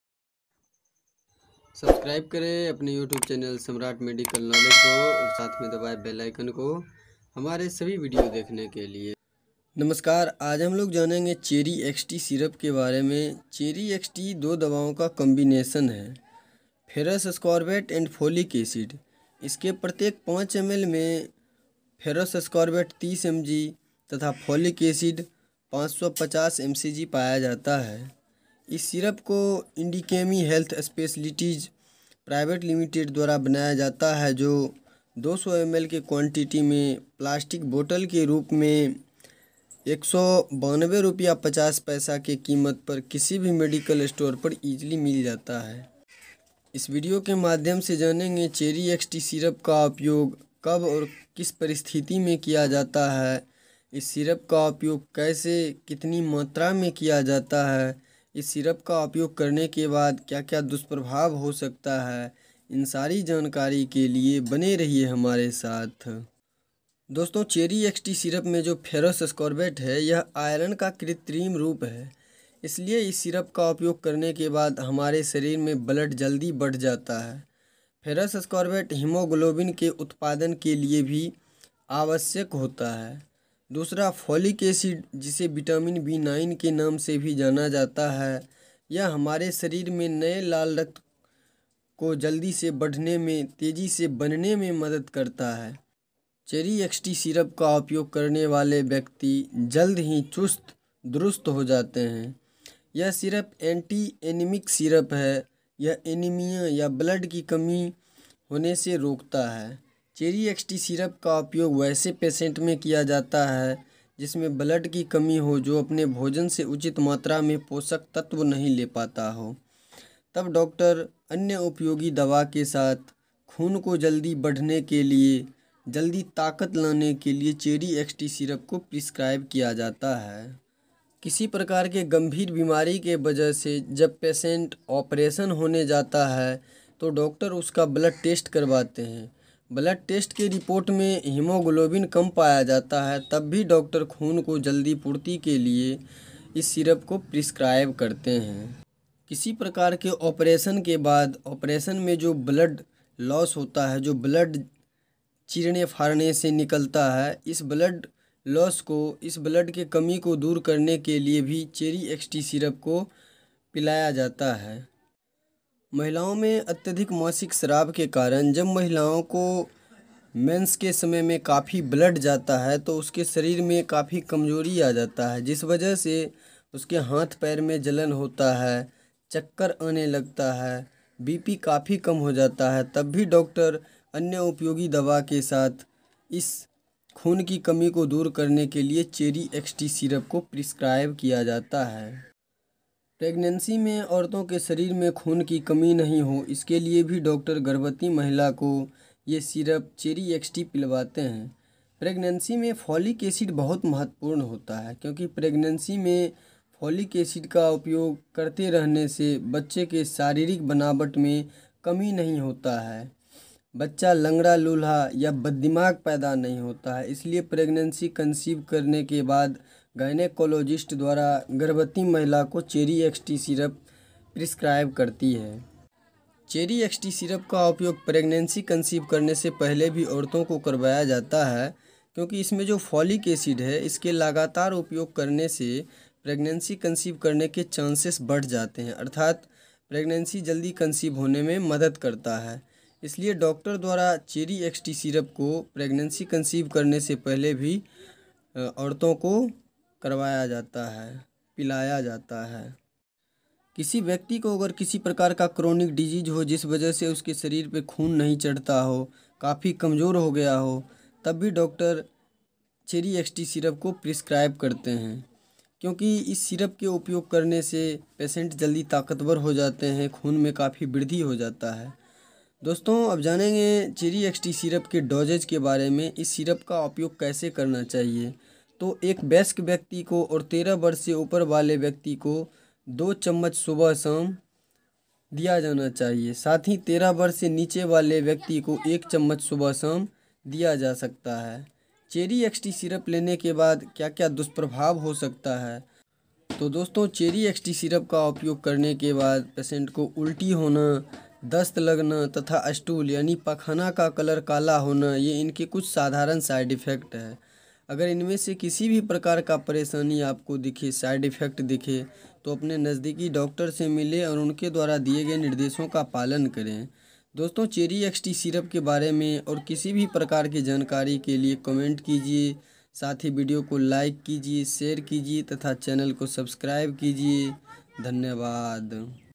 Cheri xt syrup in Hindi. खून की कमीं दूर करें। uses । doses । side effects।
Cheri xt syrup
composition - ferrous ascorbate 30mg folic acid 550mcg
uses l doses l side effects l mrp l brand l full review in Hindi
is video mein cheri XT syrup ke bare mein pura jankari Diya gaya hai. Jaise iska upyog kab Kiya jata hai. kitni Matra mein kiya jata hai. kitne din tak Kiya jata hai. tatha is syrup ka upyog karne ke bad kya kya dusprabhav ho sakta hai.

Cheri xt syrup in Hindi
Cheri xt syrup uses in Hindi
Cheri xt
Cheri xt suspension